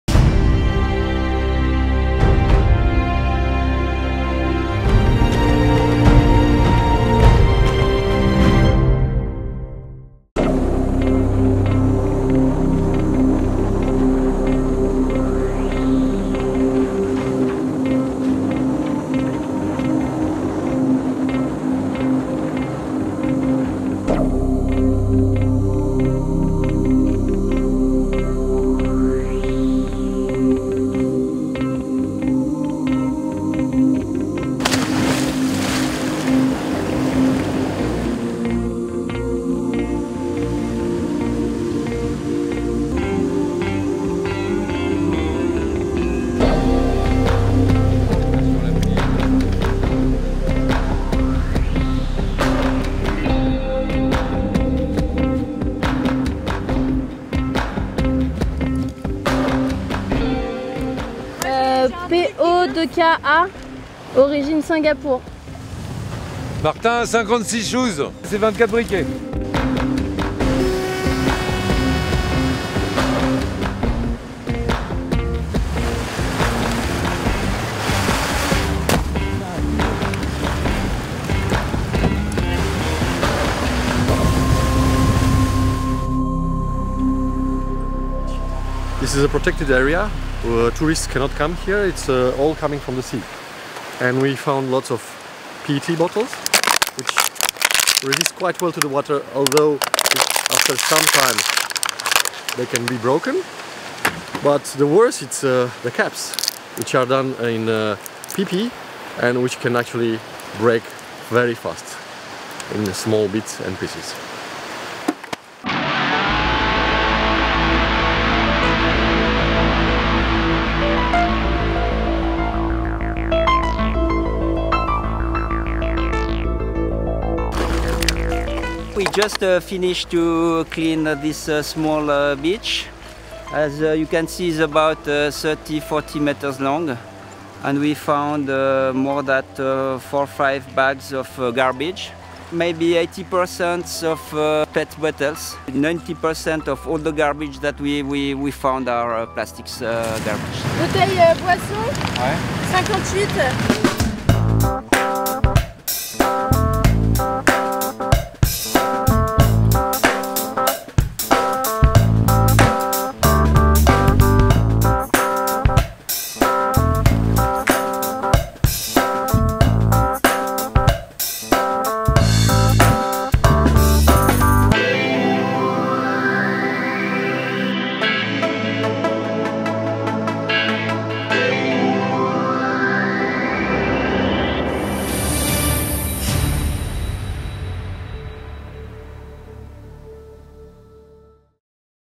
ODYSSEY 2015 - Palau : Stopover in Koror
The Race for Water Odyssey teams stopped on Koror (Palau), located in the Western Pacific Ocean, for a last scientific stopover in the Pacific. In this remote archipelago, plastic pollution brought its share of surprises: all sorts of macro debris were scattered in every locations while micro plastics were nearly absent.